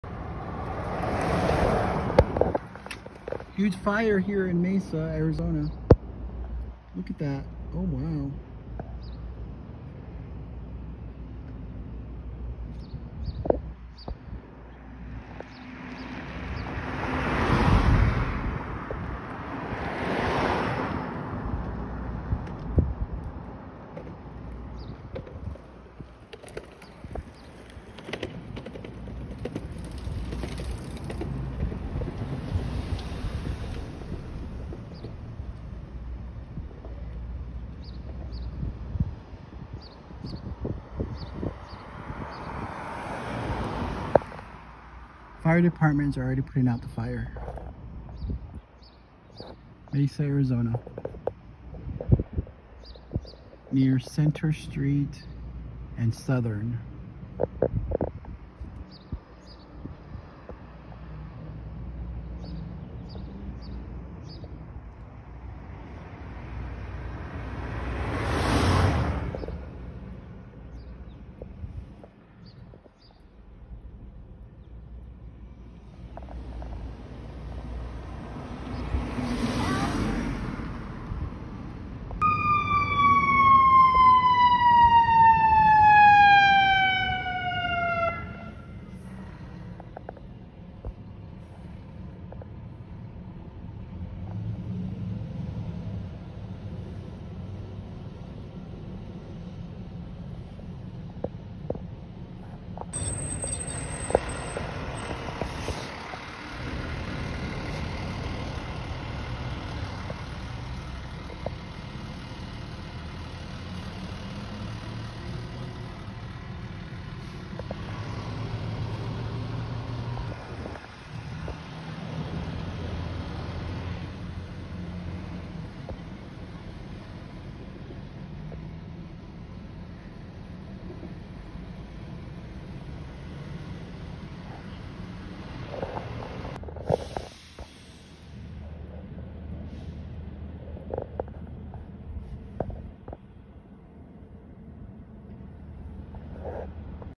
😱Gun shop on fire in Mesa AZ 😱
😱Gun shop on fire in Mesa AZ near Center st / Southern Ave at 09AM on Memorial Day Monday May 30th 2022 😱